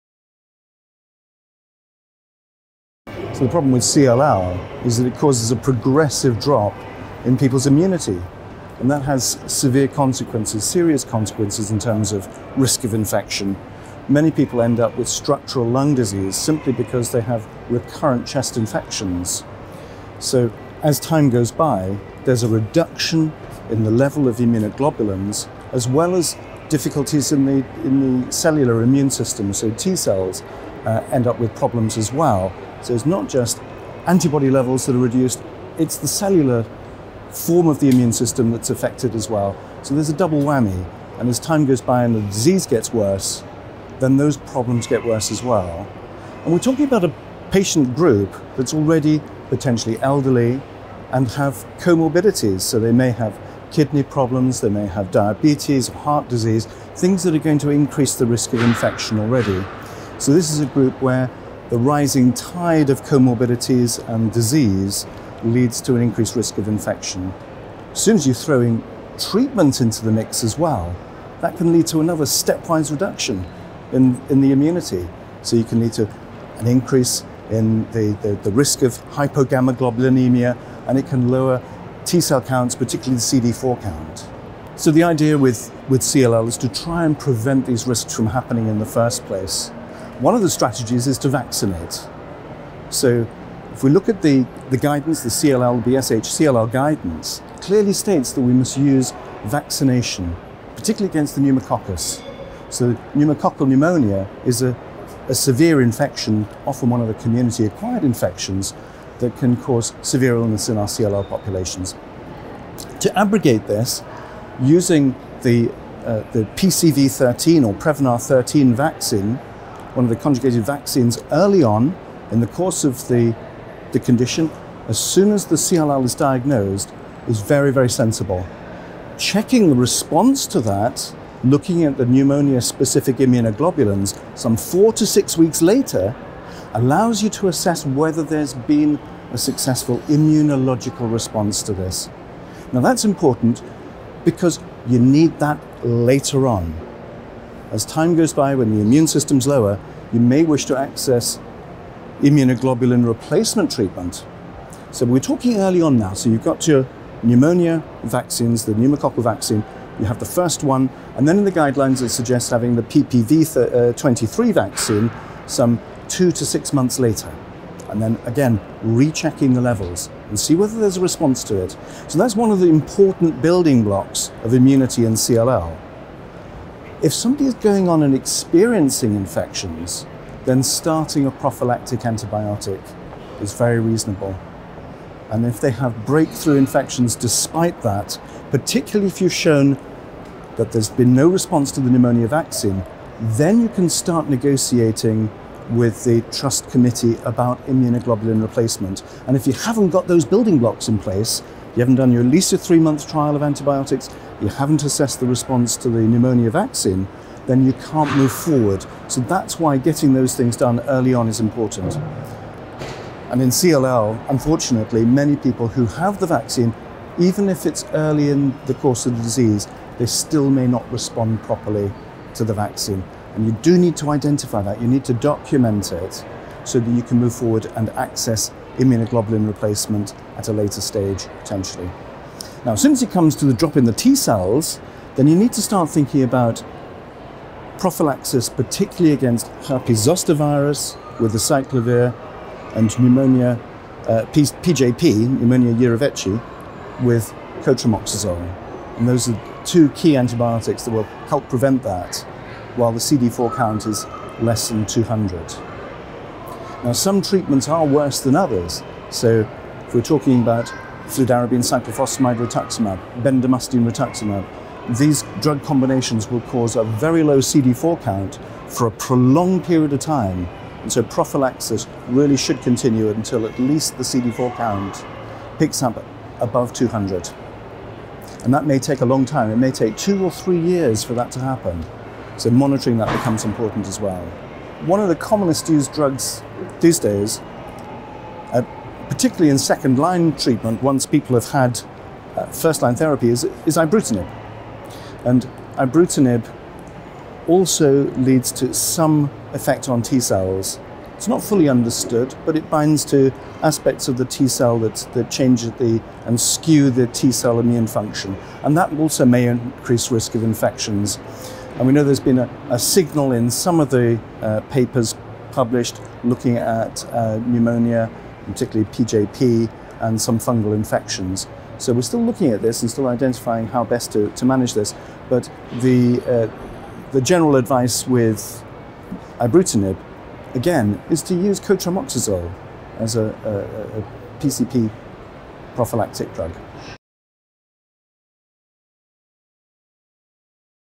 Infectious complications of CLL: prevention and treatment
Ben Kennedy, MB ChB, FRCP(UK), FRCPath, University Hospitals of Leicester NHS Trust, Leicester, tells us about the infectious complications of chronic lymphocytic leykemia (CLL) and the agents commonly used to treat it, highlighting what can be done to both avoid and address these issues. This interview took place at the British Society for Haematology (BSH) 2019 Annual Meeting, held in Glasgow, UK.